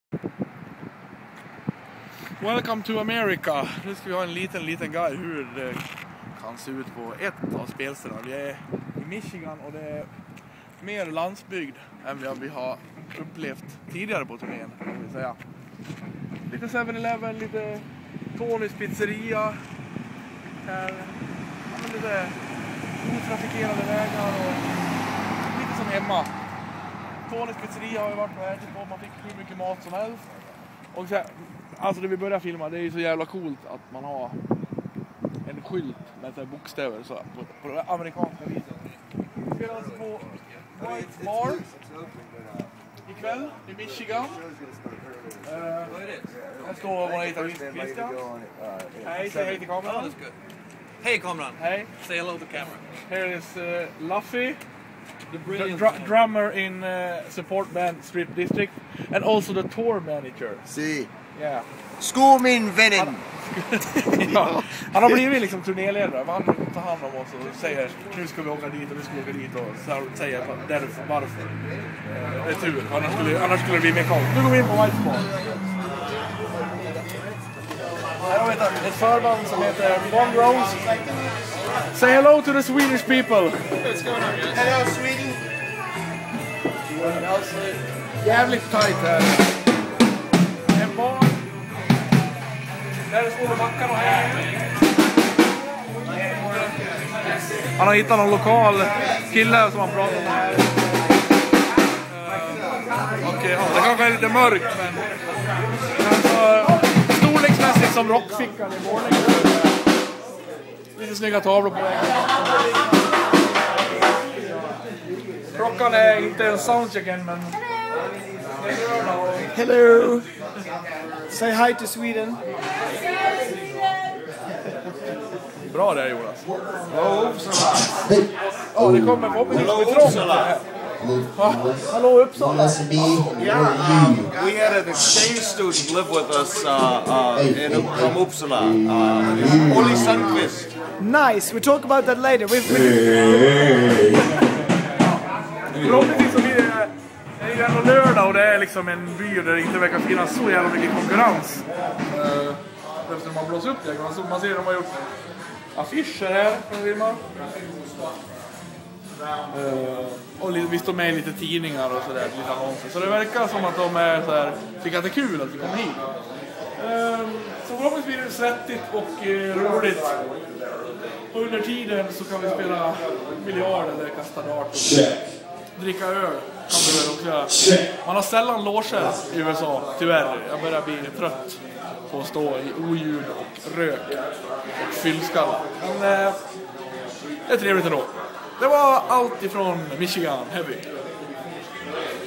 Toxic Lab Rats på White's bar
En improviserad guide på White's bar i Michigan